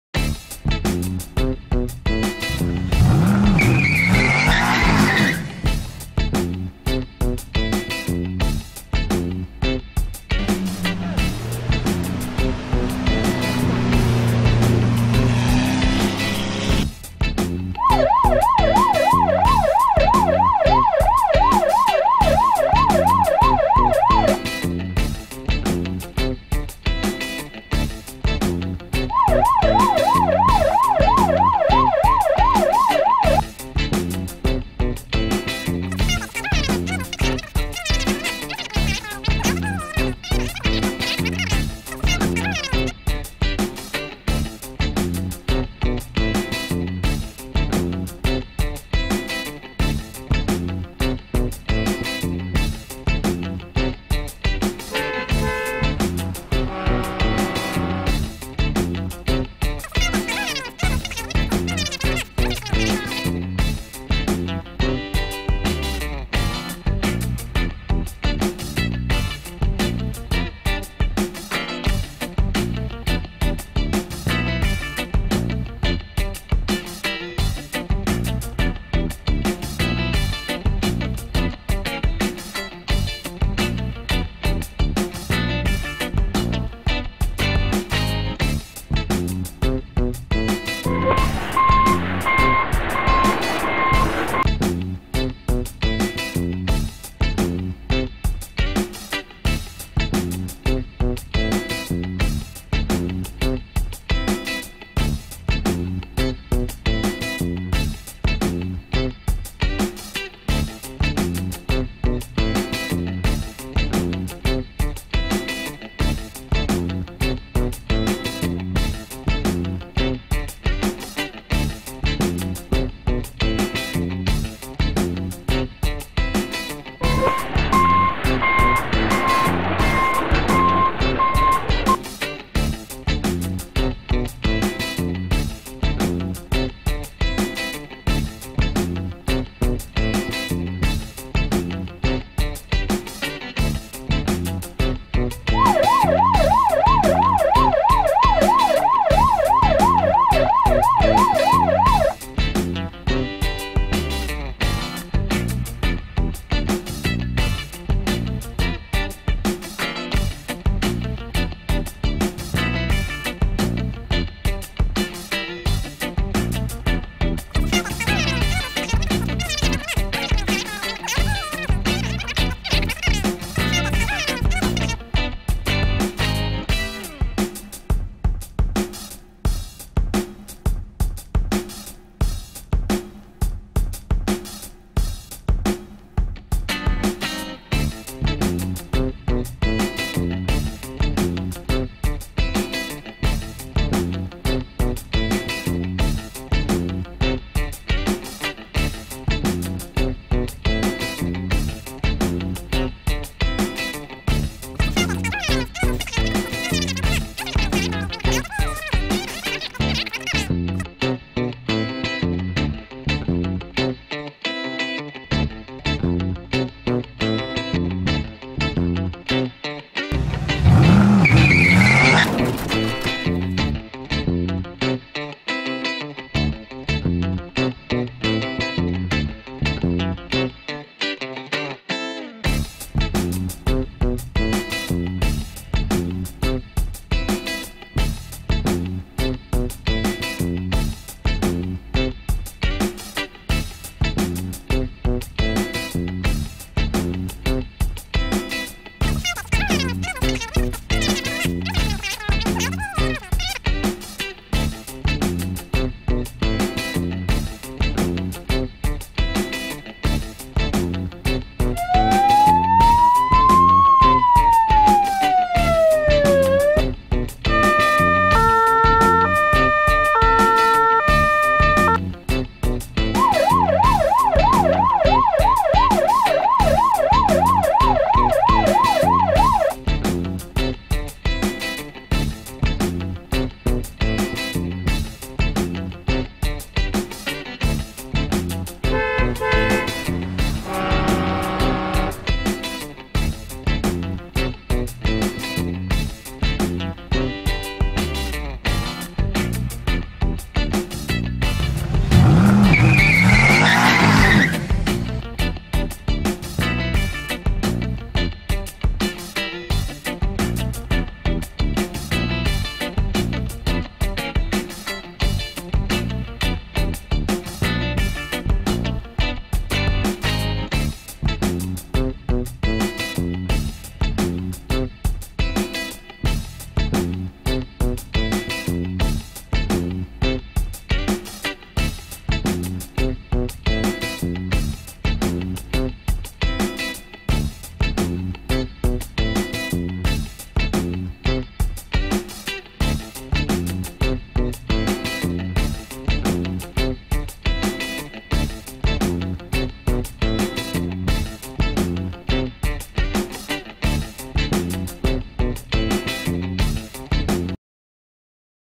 Blaze Haciendo Carreras de Coches, jugamos y nos divertimos mucho.
Hoy jugamos con Blaze hacer Carreras de Coches, aunque tenemos algun problema con La Ley jeje, Espero que les guste el Video.